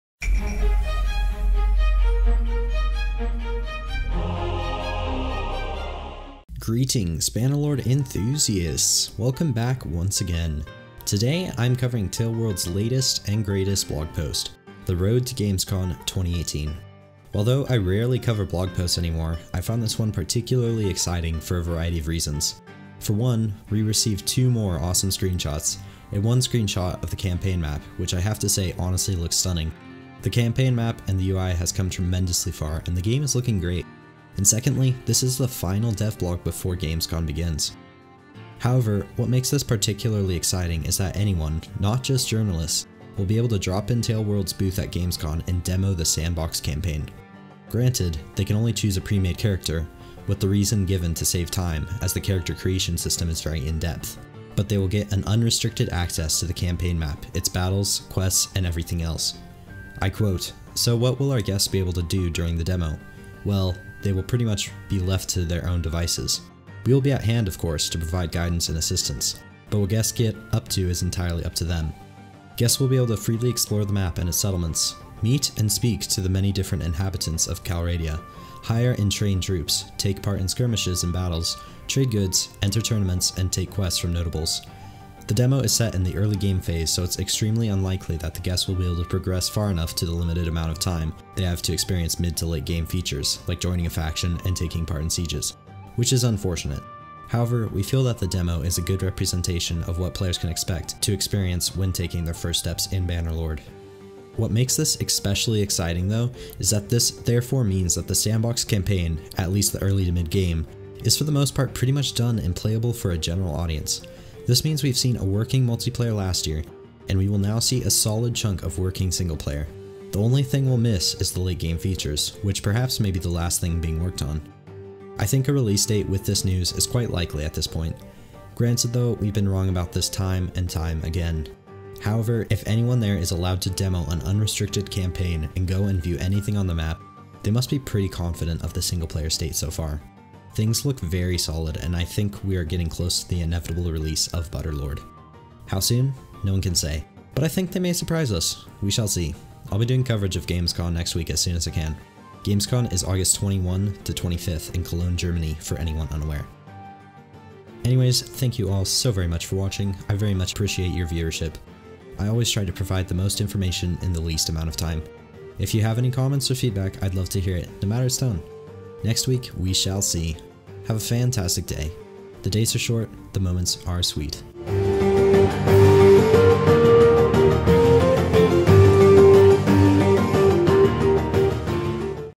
M&B: Bannerlord There's a Playable Demo at Gamescom 2018!
Hey guys! Brand new blog post from Taleworlds detailing some exciting news for their upcoming showcase of Bannerlord at Gamescom 2018 in Cologne, Germany August 21-25.